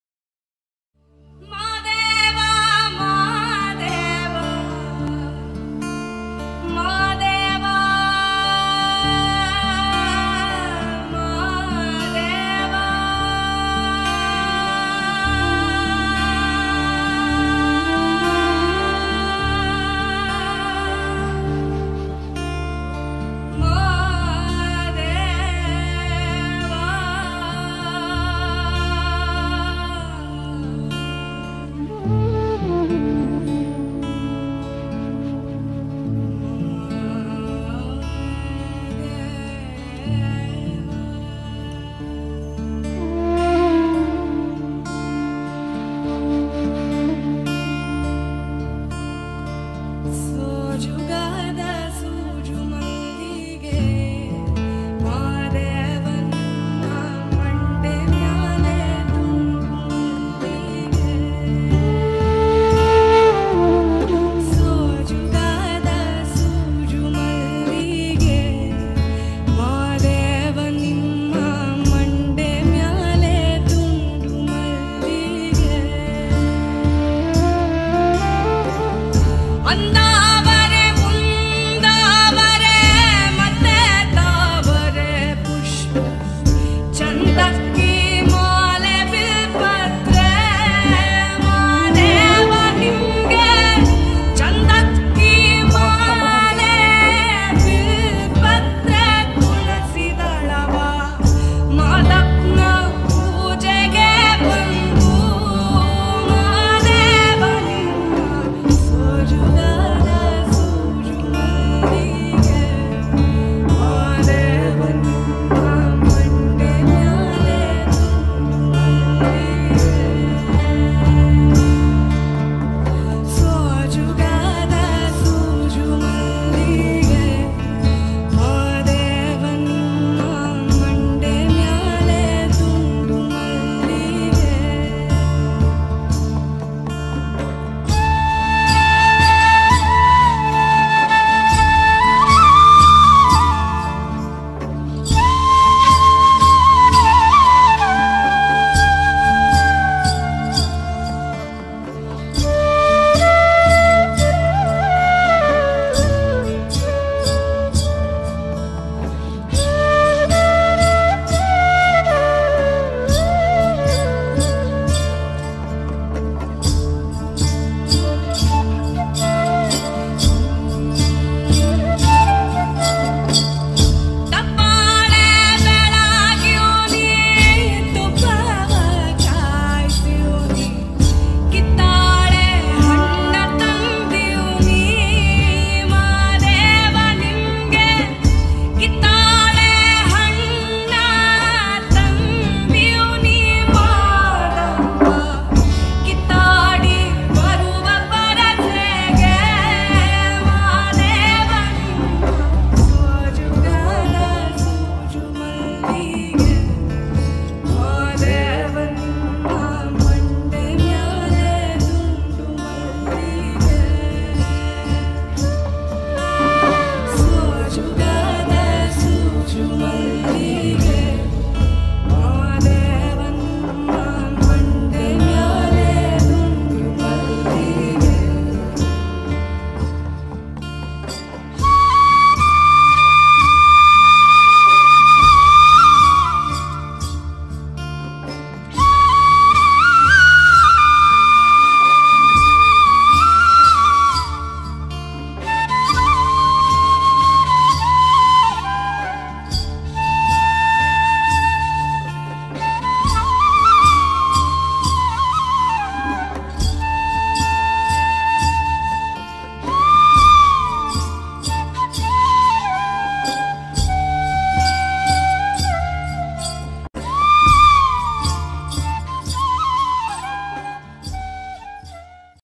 Painting of ADIYOGI SHIVA || Timelaps video || PANCHU ART'S
@isha @isha foundation @sadhguru @ananyabhat @sounds of isha
Fast painting of ADIYOGI SHIVA Timelaps video